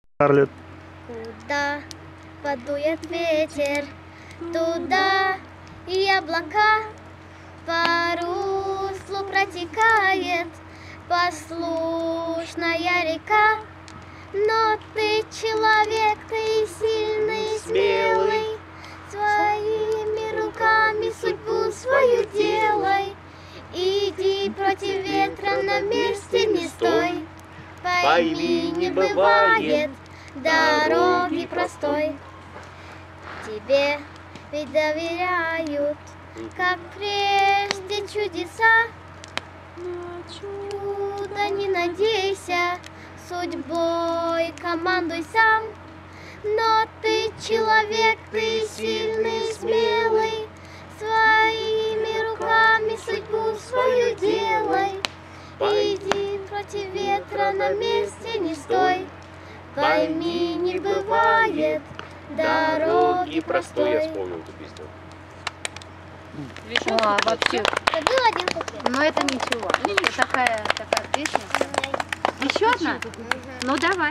Canadian Rockies Camp 2018 - 9
Members of the Alberta Interscience Association and Accredited Russian School of Edmonton participated in our annual Canadian Rockies Camp 2018 on May 18-21, 2018 near Cline River and Abraham Lake, at David Thompson Resort on the David Thompson Highway (#11), a 4.5 hour drive from Edmonton. We had fantastic weather and an unforgettable time. See you next year!!!     Enjoy: